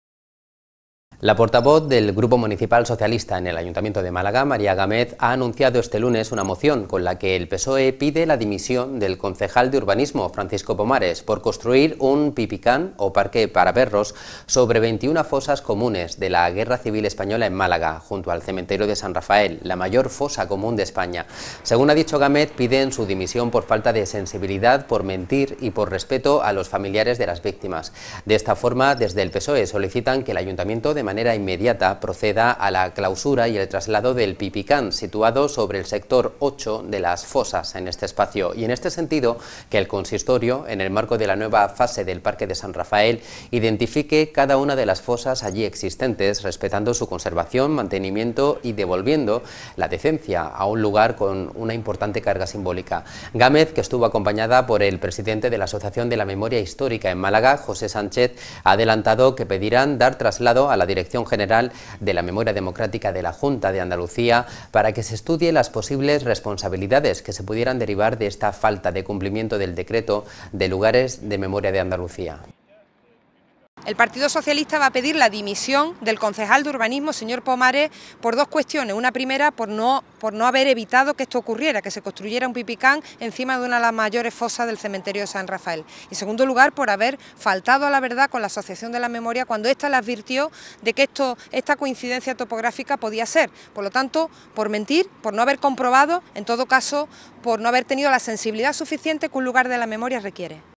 Málaga 24h TV - PSOE pide la dimisión del concejal de urbanismo de Málaga por el Pipican sobre fosas
La portavoz del Grupo Municipal Socialista en el Ayuntamiento de Málaga, María Gámez, ha anunciado este lunes una moción con la que el PSOE pide la dimisión del concejal de Urbanismo, Francisco Pomares, por construir un pipican o parque para perros sobre 21 fosas comunes de la Guerra Civil española en Málaga, junto al cementerio de San Rafael, la mayor fosa común de España.

Según ha dicho Gámez, piden su dimisión por falta de sensibilidad, por mentir y por respeto a los familiares de las víctimas.

English:

The spokesperson Municipal Socialist Group in the city of Málaga, María Gámez, announced Monday a motion with which the Socialist Party calls for the resignation of Town Planning, Francisco Pomares, to build a pipican or dog park about 21 mass graves the Spanish Civil War in Málaga beside the cemetery of San Rafael, the largest mass grave in Spain.

Gamez has said, calling for his resignation due to lack of sensitivity, for lying and respect for the families of the victims.